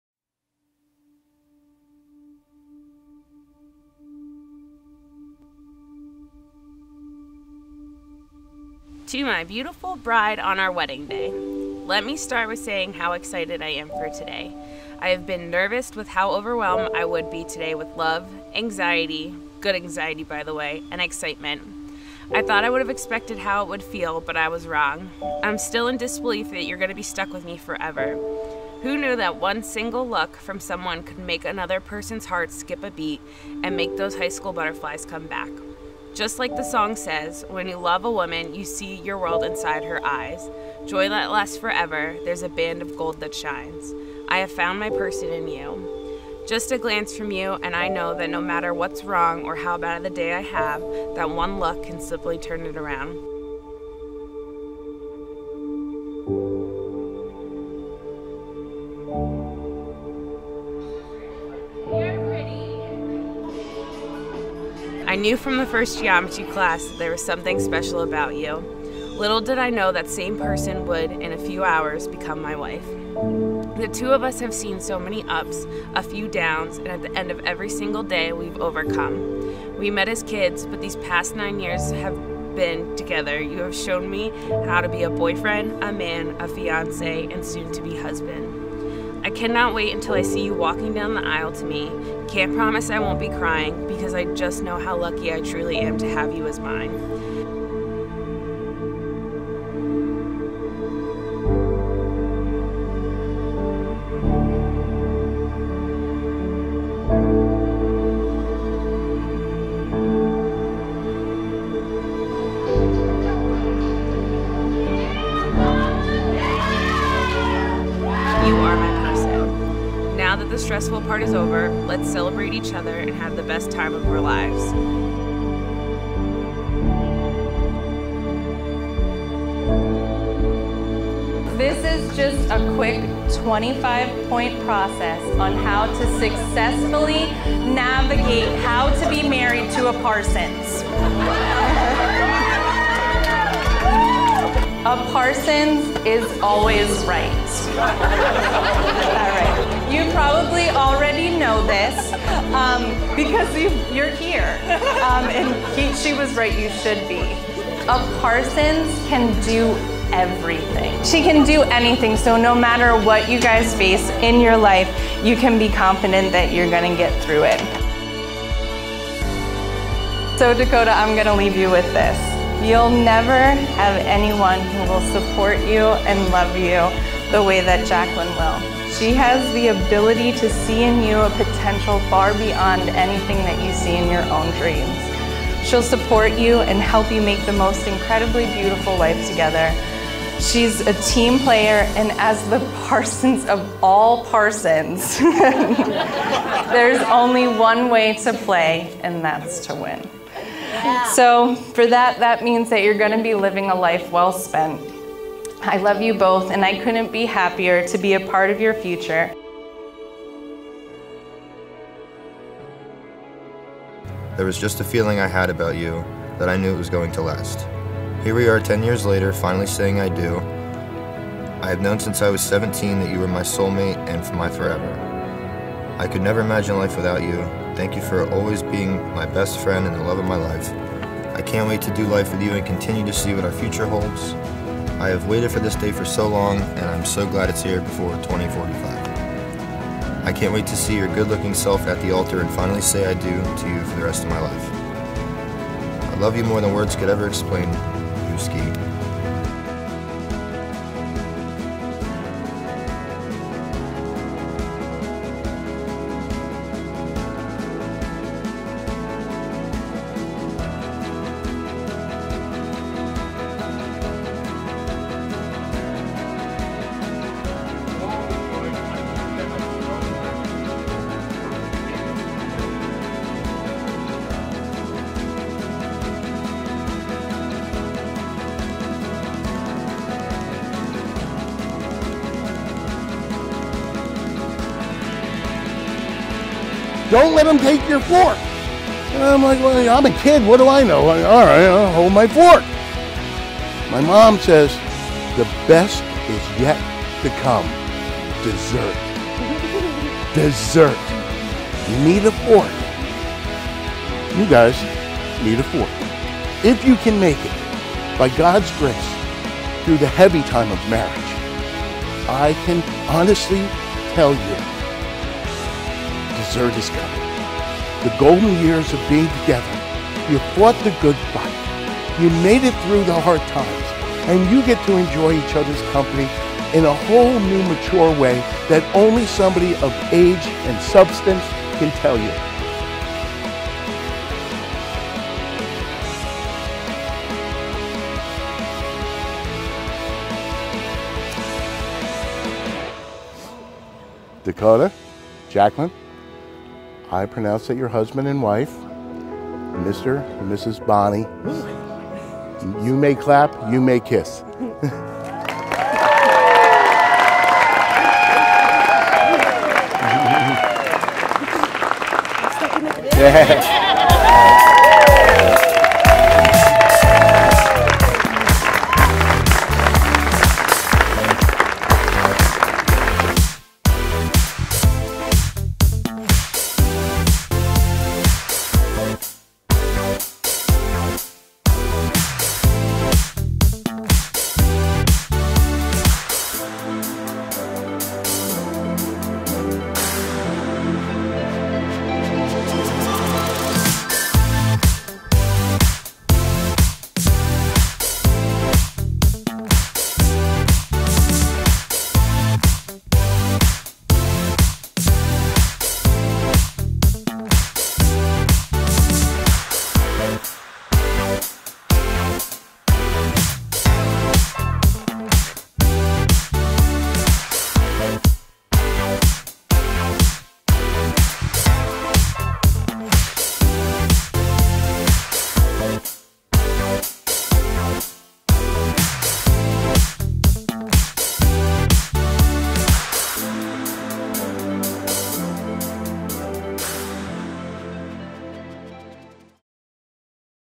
Jackie and Dakota
We loved spending the day with Jackie and Dakota. You couldn't ask for a more lovely September day, a more picturesque location than Silver Birches at Lake Wallenpaupack, or a more fantastic crowd of friends and family!

The ceremony was held at the edge of the lake with gorgeous views right before sunset.

As soon as the sun set and beillies were full, the party started. We have known this family for a long time but we have never seen them celebrate as hard as they did for Jackie and Dakota. We are talking, tearing down the dance floor, air guitar solos, dancing on the tables, and tears-of-laughter inducing toasts. It was such a beautiful thing to be a part of!

Jackie and Dakota were high school sweethearts. They met in an 11th Grade Geometry class and have been together for 9 sweet years. They have been running a food truck business along with their friend and business partner Dustin together for the past 3 years. Dakota is an amazing chef and Jackie manages the business. After almost a decade together they make an awesome team!

We join their families and friends in wishing them all the best things this life can offer!